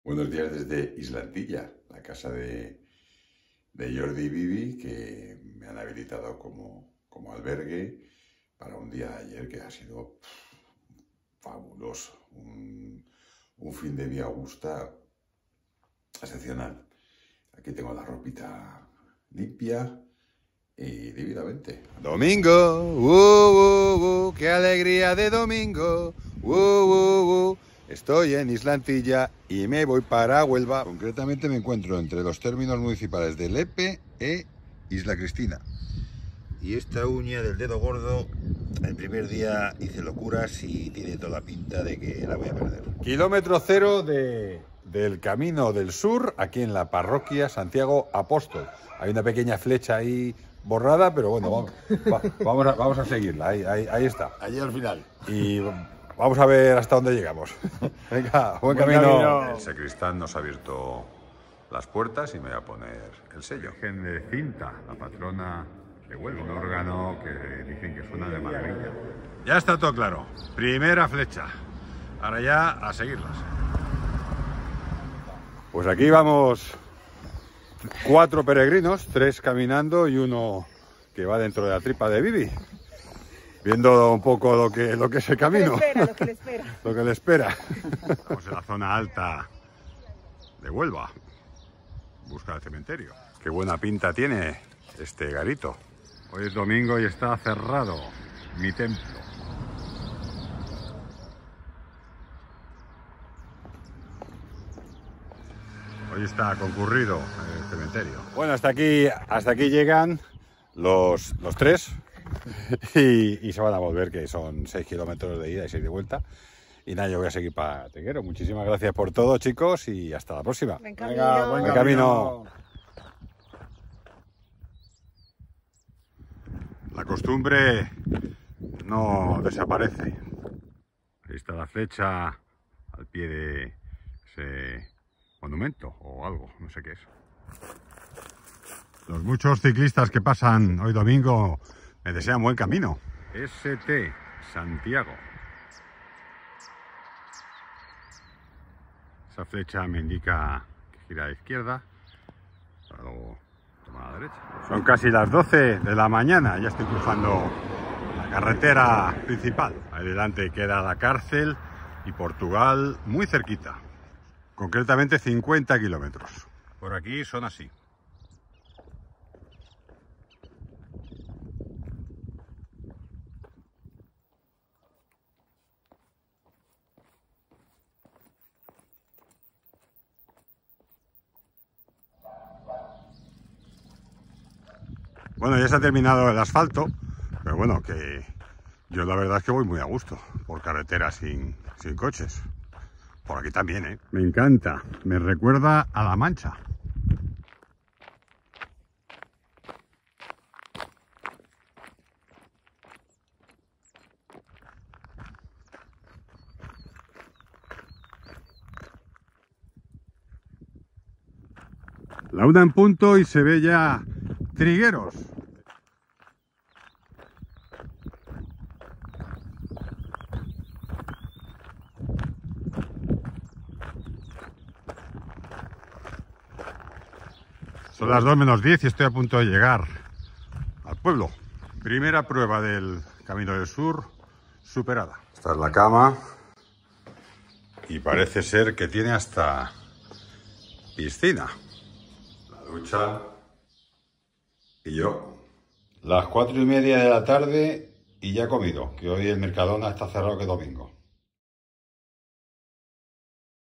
Camino 49 Del Sur Junio 2022 Parte 1
De Huelva a Trigueros (en buena compañía) el 5 de junio de 2022…